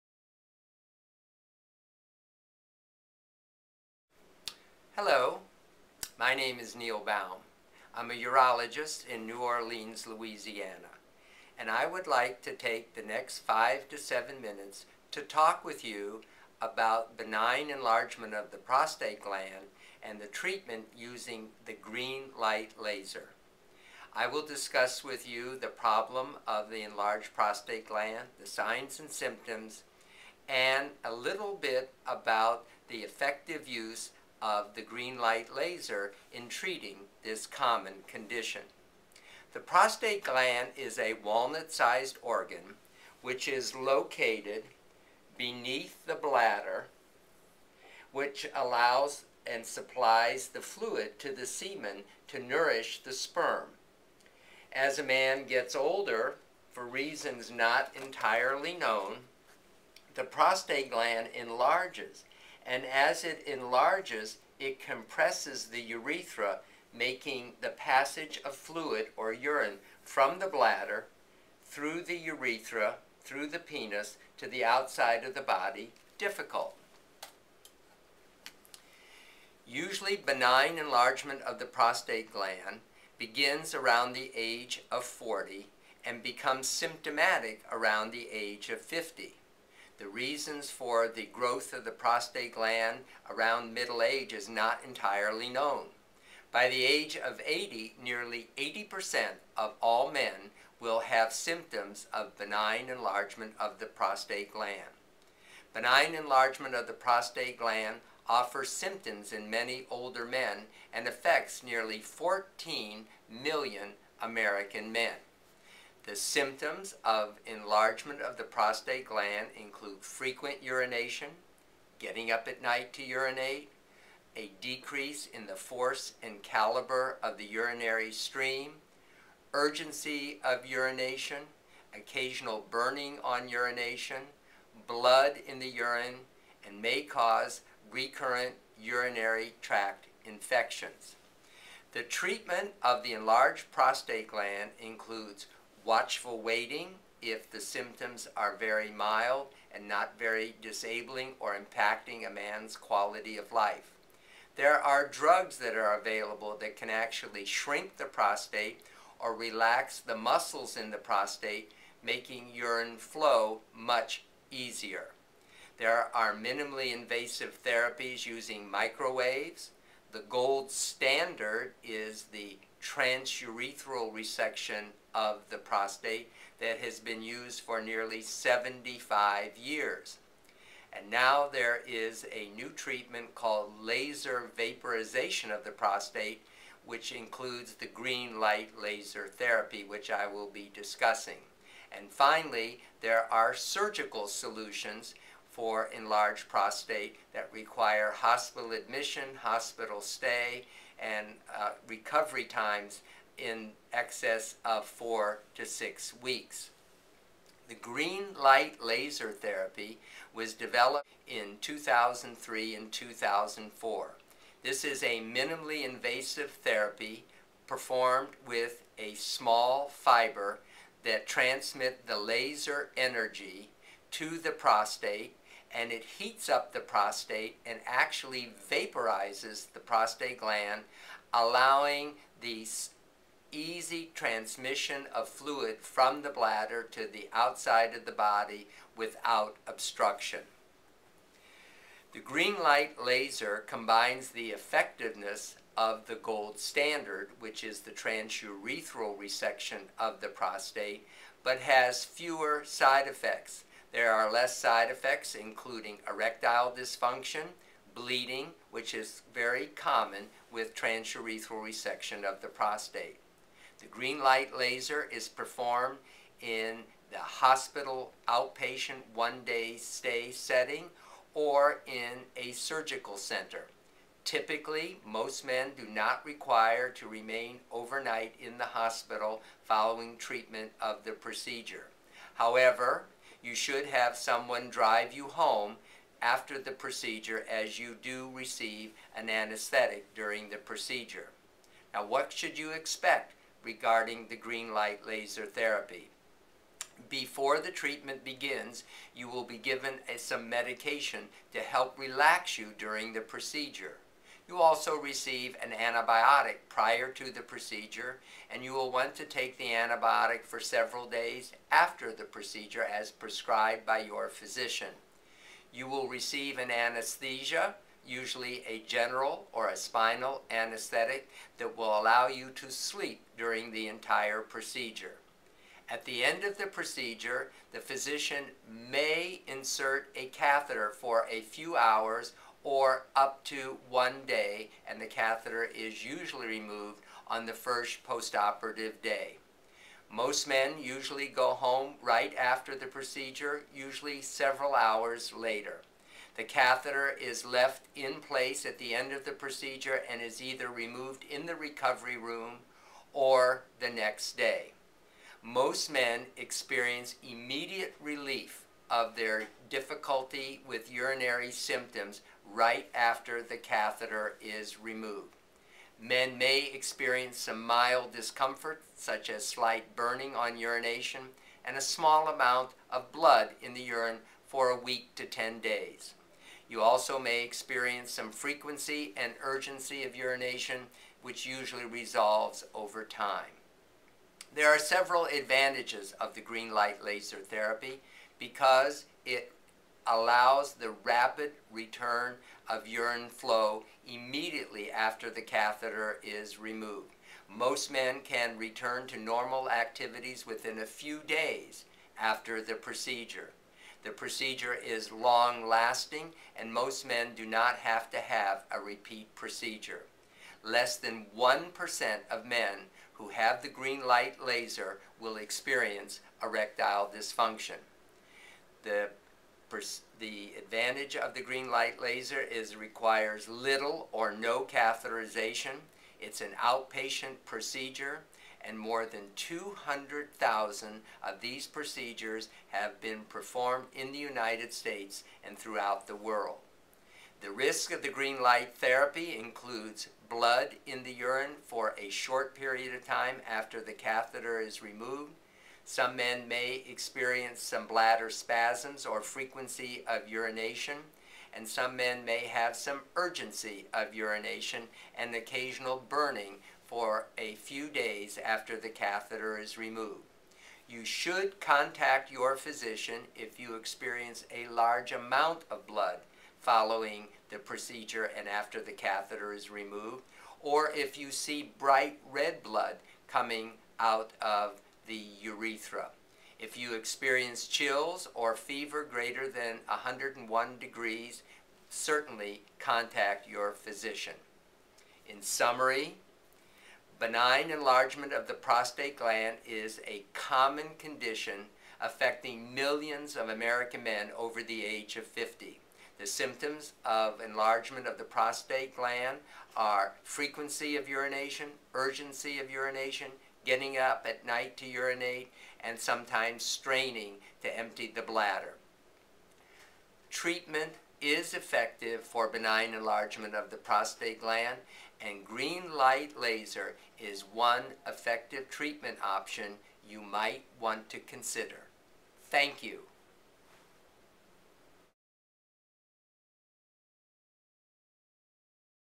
Greenlight Laser by Dr. Neil Baum
Benign enlargement of the prostate gland causes symptoms of urinating frequently, getting up at night to urinate, and urgency of urination.  Treatment options include medications, microwave therapy, surgery, and now the use of lasers to remove the obstructing prostate tissue.  The treatment is done without hospitalization and usually does not require a catheter after the procedure.